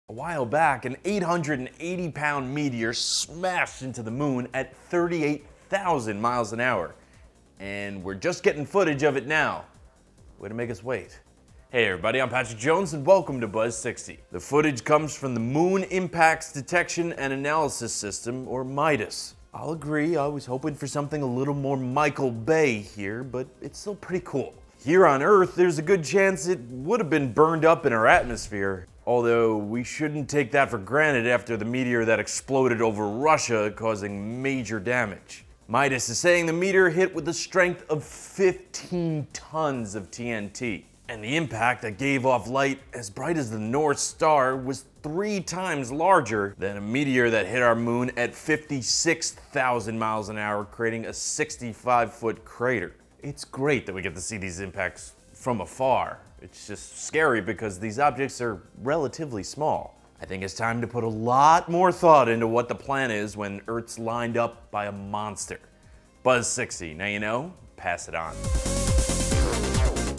Meteor Slams Into Moon with Force of 15,000 Tons of TNT
The Moon Impacts Detection and Analysis System released video of a major collision of a meteor hitting our moon at about 38,000 miles per hour. The explosion released a light as bright as the North Star, Polaris which is scary from only an 880 lb. object says Patrick Jones (@Patrick_E_Jones).